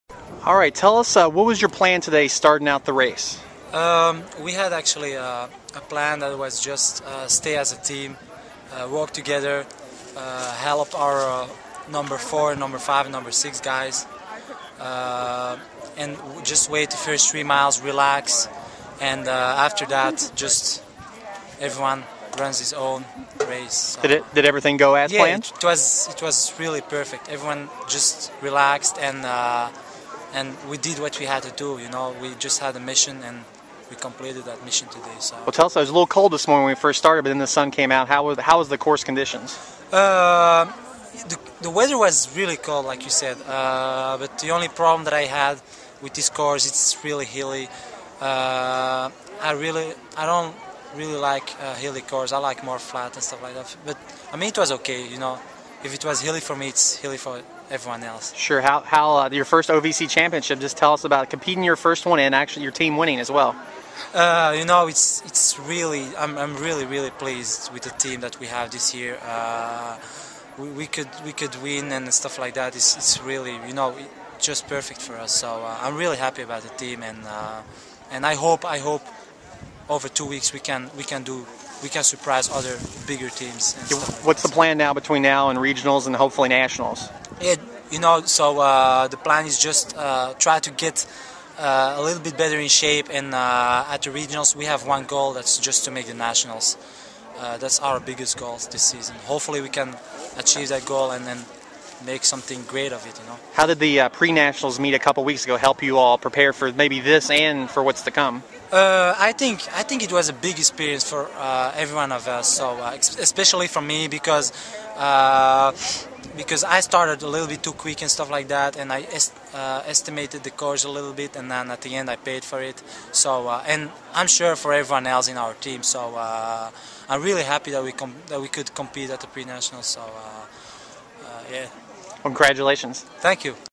2010 OVC Cross Country Championships -- Soufiane Bouchikhi
Eastern Kentucky University freshman Soufiane Bouchikhi is interviewed after winning the 2010 OVC Cross Country Championships.